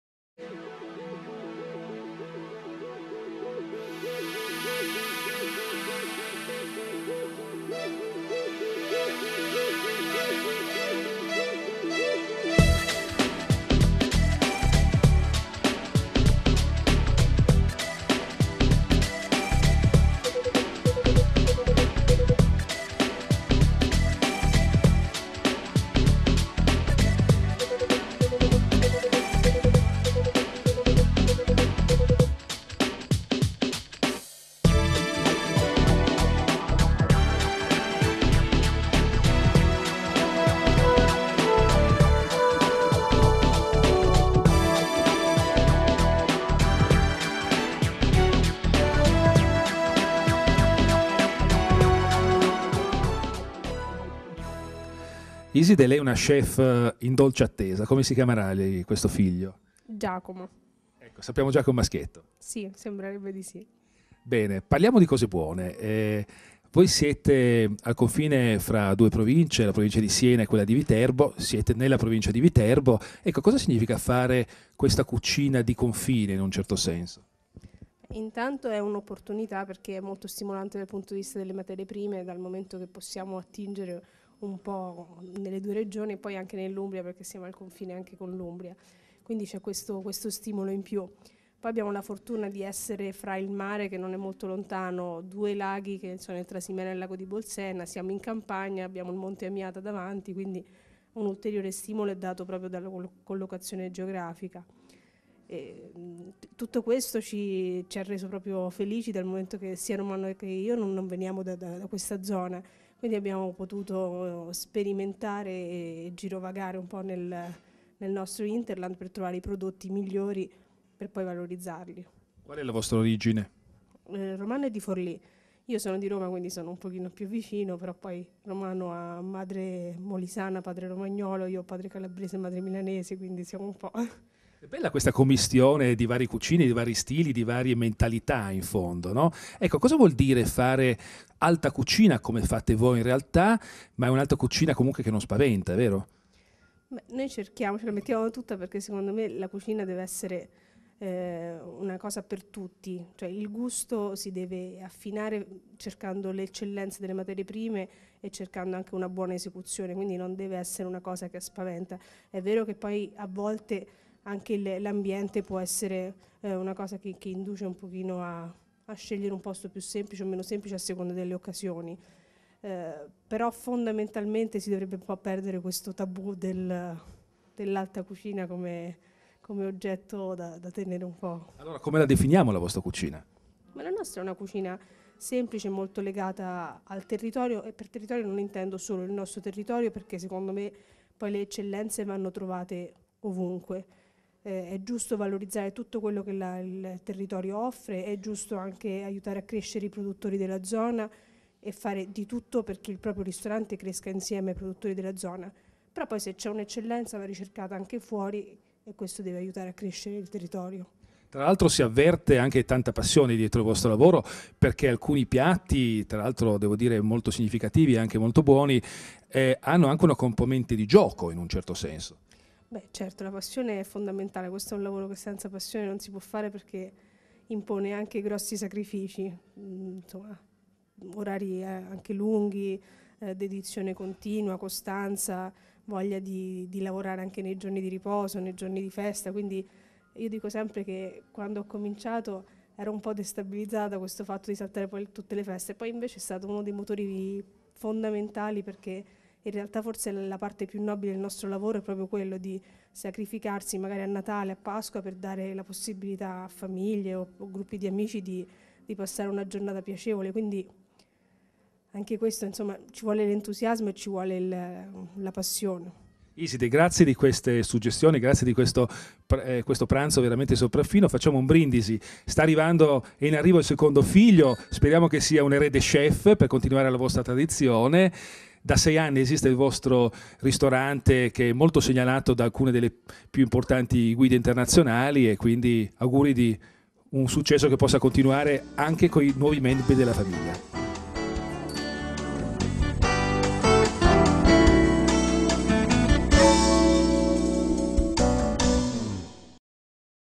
LA PAROLINA, ristorante a Trevinano, Viterbo
(ediz.02/2011) - Nel periodo autunnale si possono assaggiare i funghi dell' Amiata, i tartufi delle crete senesi, la cacciagione dal capriolo alla lepre, dai piccioni fino alle rinomate razze vaccine che pascolano in queste terre. Il pesce sempre fresco, proveniente sia dal vicino Lago di Bolsena sia dal mar Tirreno, viene scrupolosamente e quotidianamente selezionato presso Marta. Tutto fatto in casa dalla pasta tirata al mattarello, come la tradizione vuole, al pane servito in diversi assortimenti, ai grissini, ai dolci, ai gelati e in conclusione anche i cioccolatini e la piccola pasticceria.
La carta dei vini del nostro ristorante è stata elaborata in primis per valorizzare le migliori proposte del territorio laziale, tenendo sempre un occhio di riguardo alle novità e a piccoli produttori emergenti.
Sul campo nazionale abbiamo selezionato referenze che rappresentano la produzione vitivinicola italiana; vini che hanno appassionato gli amatori del buon bere.
Uno sguardo anche alle referenze d'oltralpe con un'attenta proposta di circa 50 champagne e vini di Borgogna e Bordeaux anche di annate che le rendono perle prezione per l'enologia mondiale, il tutto supportato da una lista di mezze bottiglie e una cantina del giorno che valorizza prodotti con un ottimo rapporto qualità prezzo.
Una curata selezione di distillati come, grappe, rum, calvados, whisky e porto dà la possibilità di ultimare il pasto degustando cioccolatini artigianali.